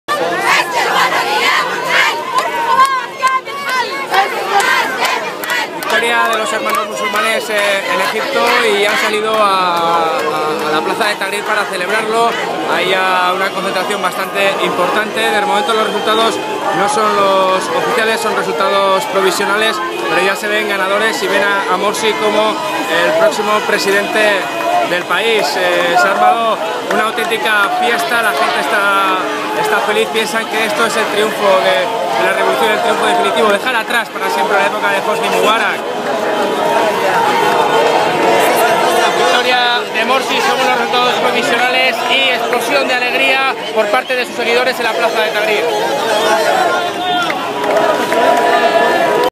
EGIPTO. Fiesta islamista en Tahrir
VIDEOBLOG. Los seguidores de los Hermanos Musulmanes celebran la victoria provisional en la urnas. Los resultados oficiales se conocerán el jueves.